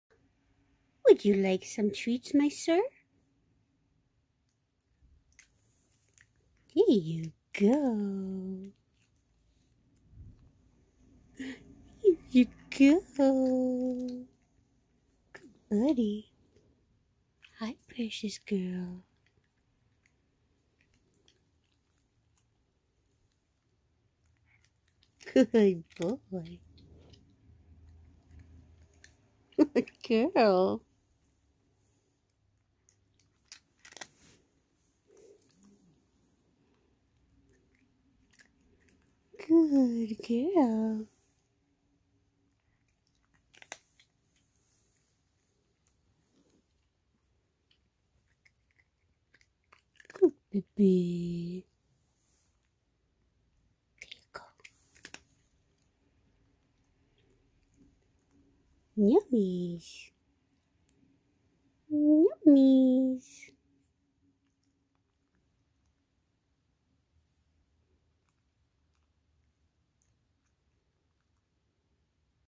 Frankie & Whisper have treats on 4.2.20
Frankie (blonde) and Whisper (black) have treats!